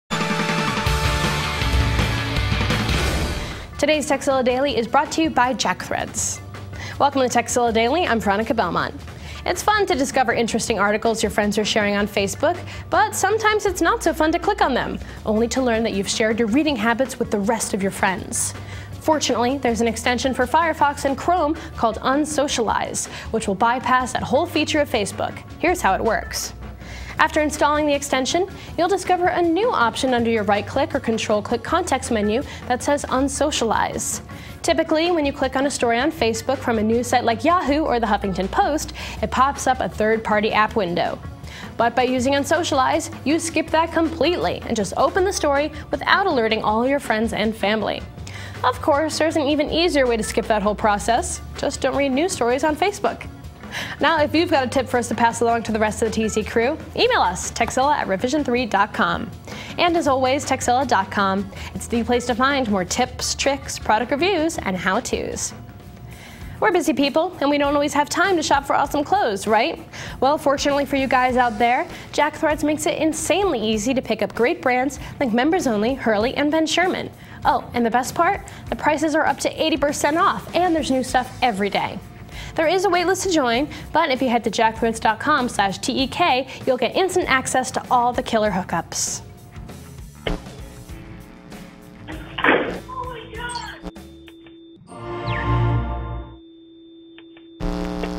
Keep Your Reading Habits Private on Facebook - Tekzilla Daily Tip
Unsocialize is a plugin that bypasses third-party prompts when reading news stories from Facebook. Read articles in peace without Facebook auto-posting your reading habits.